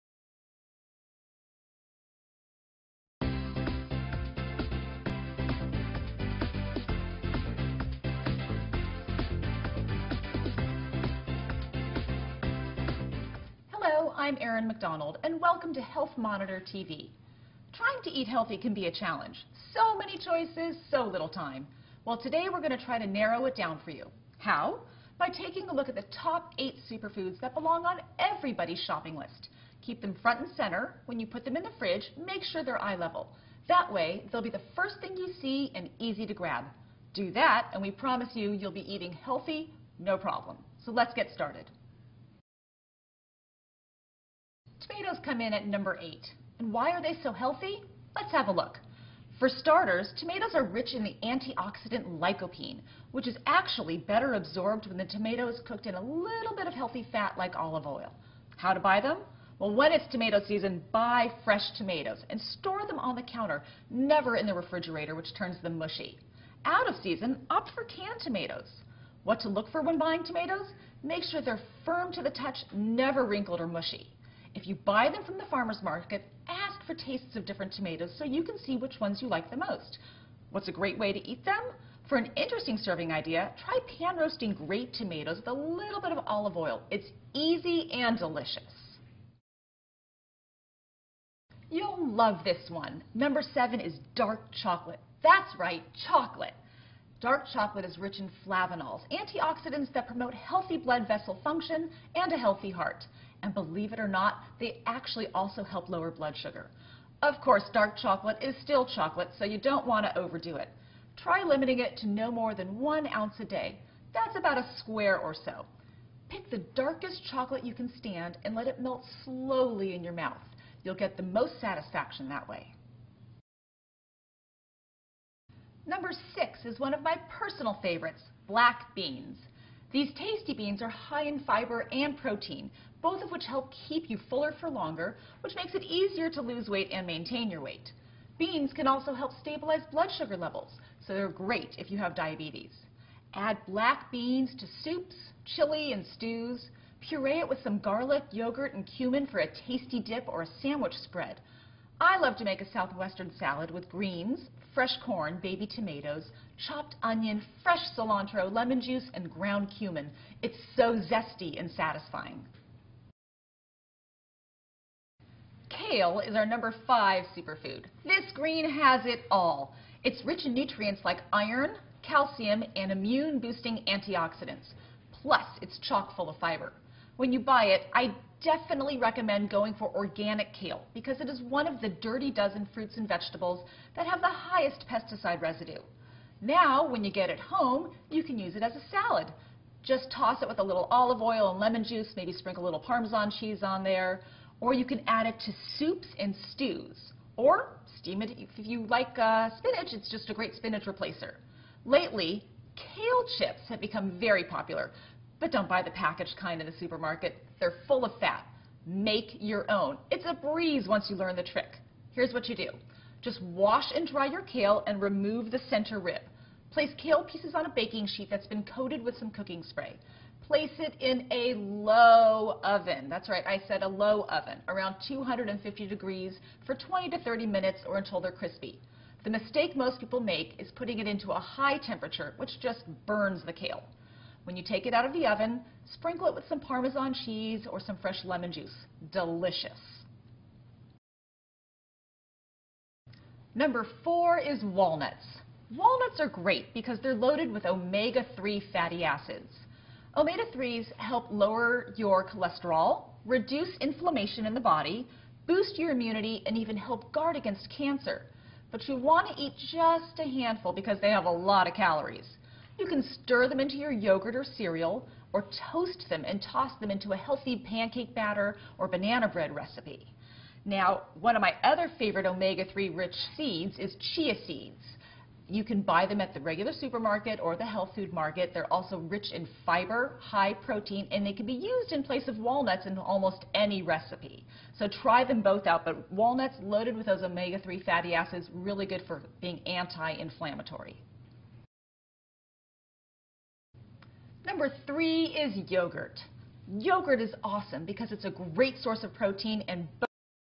Superfoods Part 1
Erin Macdonald, Nutritionist/RD, discusses the top 8 superfoods that belong on your shopping list.